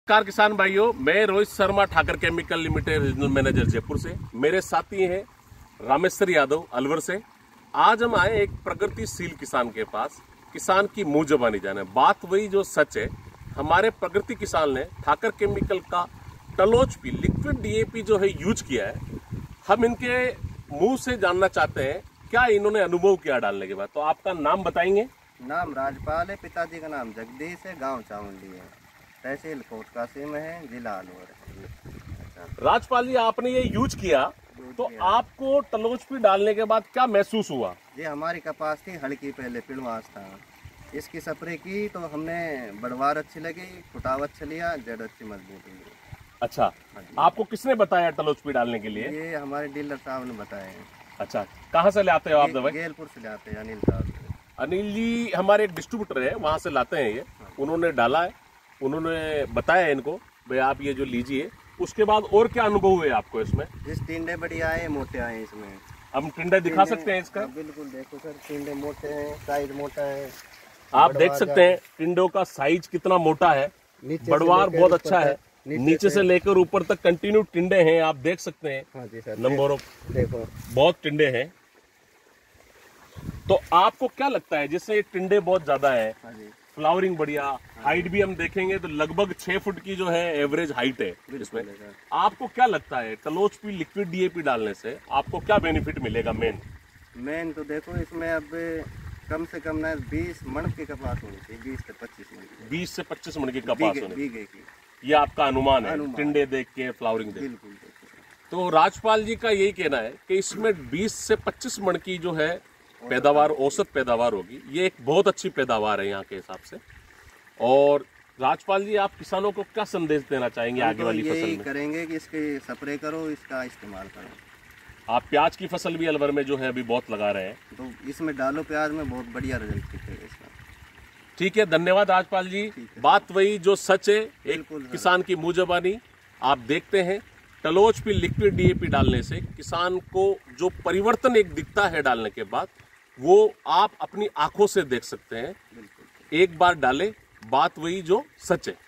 Talos-P Testimonial | टैलोस-पी की सफलता की कहानियाँ: असली किसान, असली परिणाम!
देखें कि कैसे हमारे उत्पाद ने उनकी कपास की फसल को बदल दिया है, जिससे गुणवत्ता और मात्रा दोनों में वृद्धि हुई है। अपनी फसलों के विकास में इस क्रांतिकारी उत्पाद को शामिल करने से न चूकें! 🍀

आज ही टैलोस-पी लाएँ और अपनी फसल का स्तर बढ़ाएँ! 🚀🌟

अधिक जानकारी के लिए, हमारी वेबसाइट पर जाएं।